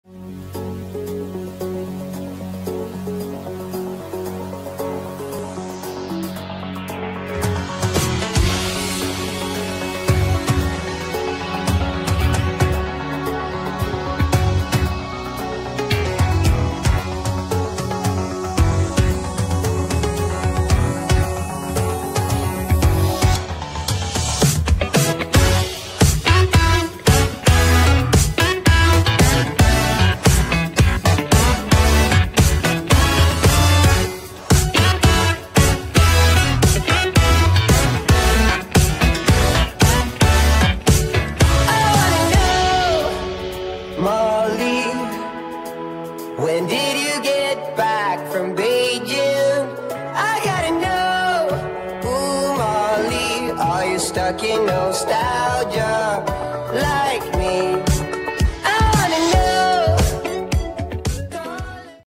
Looks for over 40 year young hair styles and lipsticks!
Our world is full of colors!  Trying long hair, short , curly and color wigs that you can  wear while wearing every color of lipsticks too! Loving the skin you are in